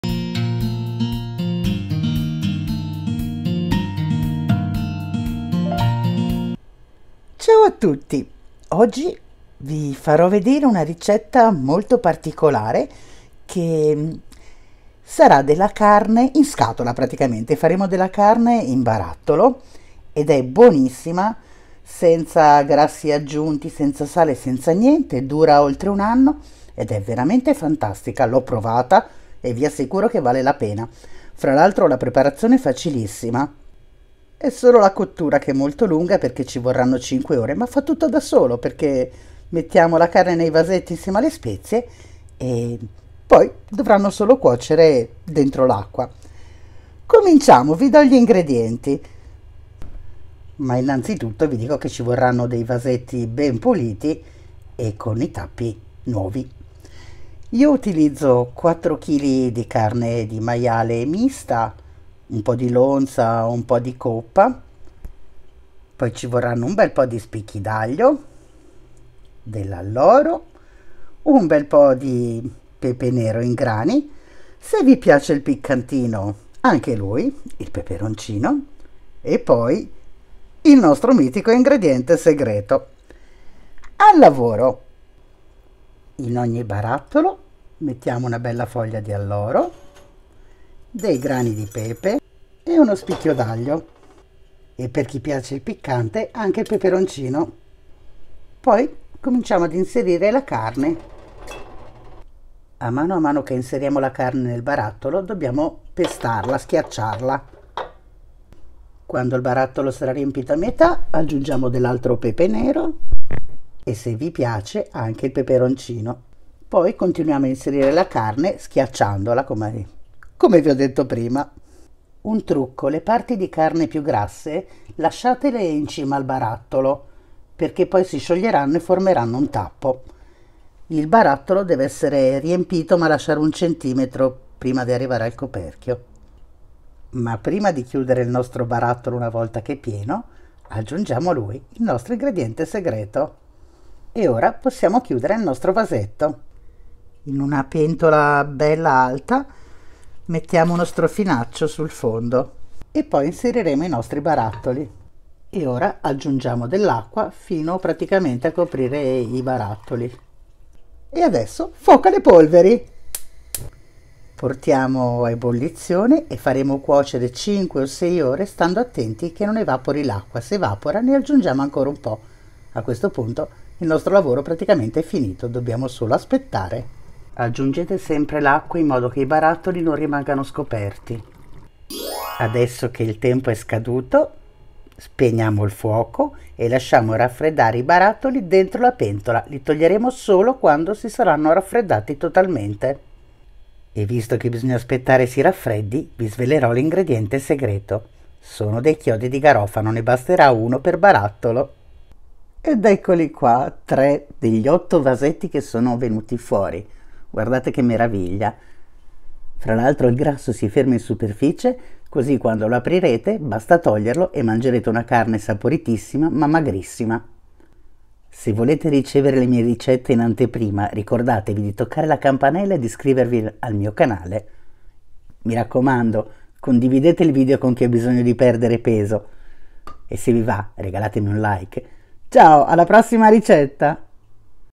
CARNE IN VASOCOTTURA LIGHT/DIETARY CANNED MEAT/CARNE ENLATADA DIETETICA
INGREDIENTI/INGREDIENTS/INGREDIENTES

CARNE MISTA DI MAIALE 4 kg
AGLIO 16 spicchi
ALLORO 8 foglie
PEPE NERO IN GRANI a piacere
PEPERONCINO PICCANTE a piacere
+ INGREDIENTE SEGRETO 8

MIXED PORK MEAT 4 kg
BLACK PEPPER CORNS to taste
HOT PEPPER to taste
+ SECRET INGREDIENT 8

CARNE MISTA DE CERDO 4 kg
AJO 16 dientes
LAUREL 8 hojas
PIMIENTA NEGRA EN GRANOS a gusto
AJI a gusto
+ INGREDIENTE SECRETO 8

Prova anche la vasocottura rapida al microonde:
Try even quick microwave vacuum cooking:
Prueba también la cocción rápida al vacío al microondas:

CARNE IN VASOCOTTURA LONZA CURRY E PERE LIGHT/MASON JAR CURRY&PEARS LOIN/LOMO CURRY Y PERAS AL VACÍO